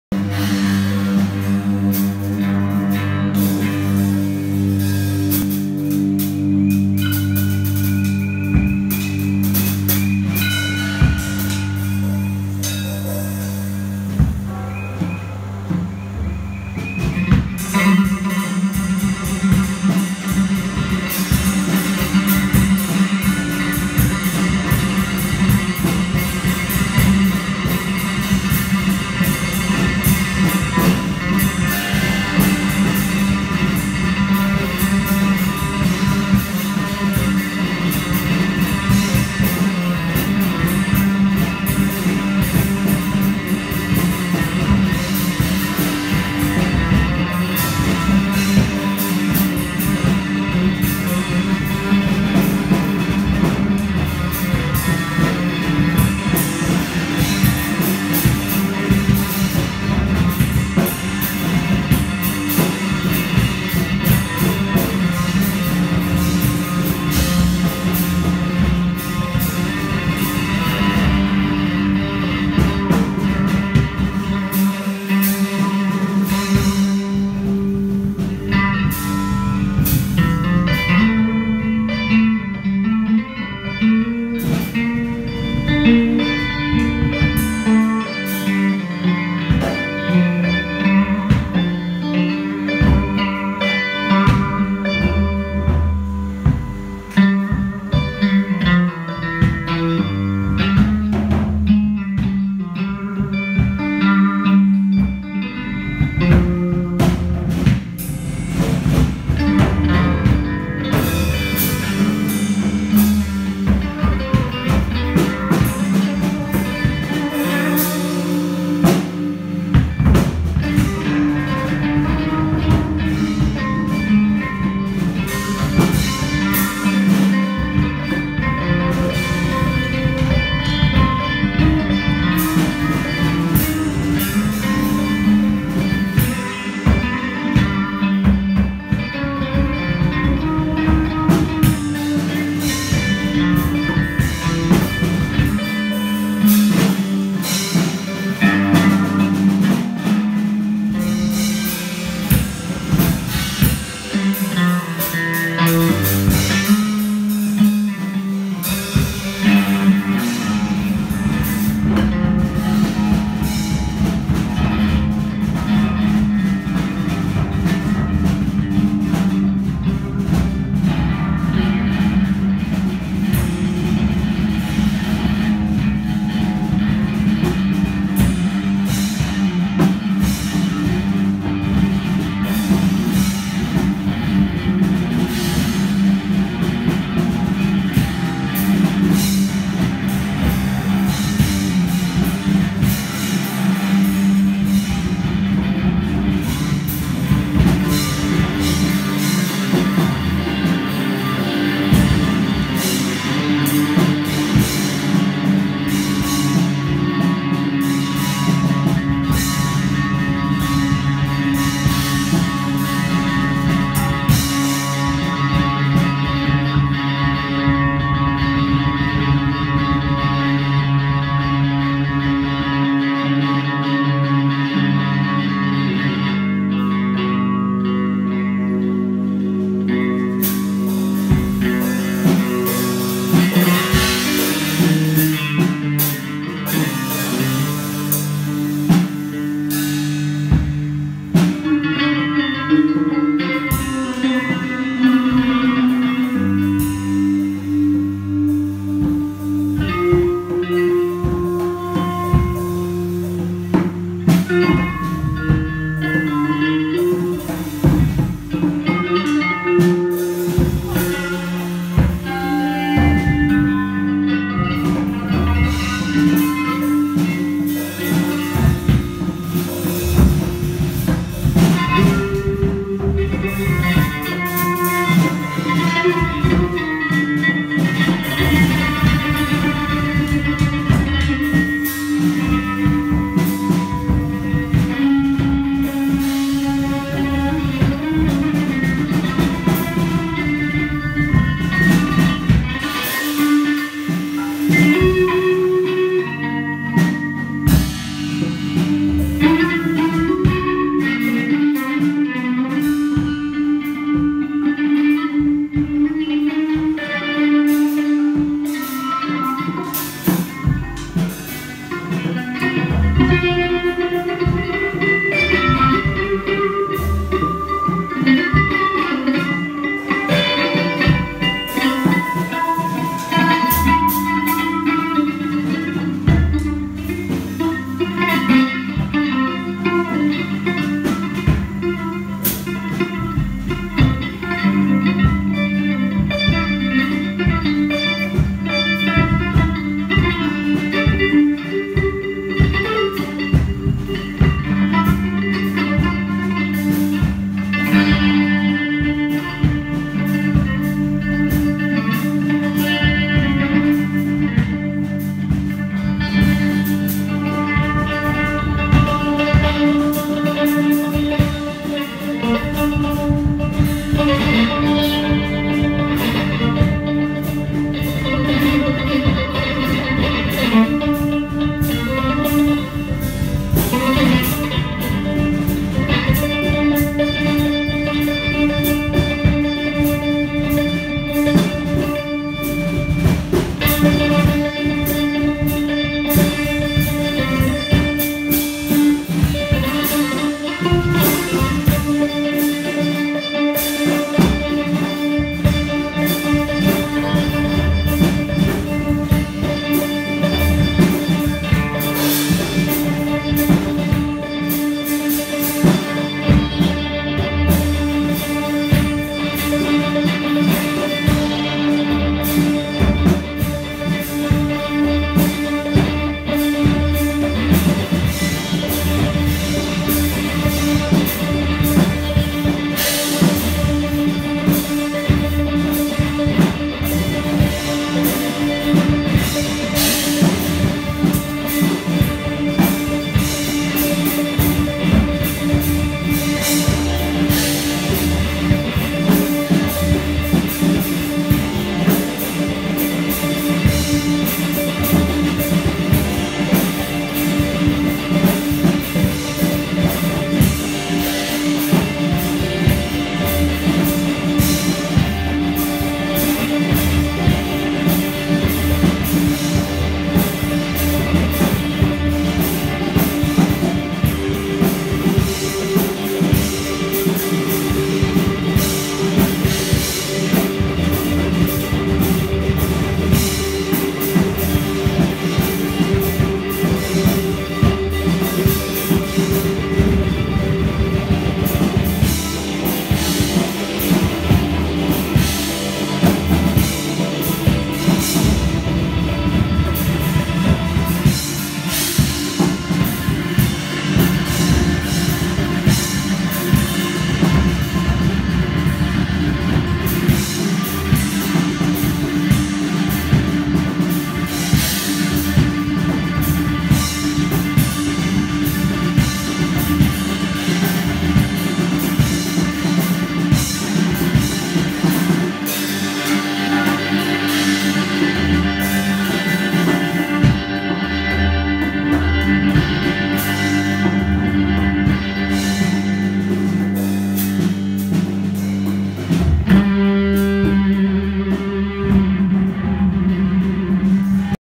Pure Enchantment 6:15:19
PURE ENCHANTMENT: Turner Williams and Ed Wilcox, at Vox Populi in Philly, 6/15/19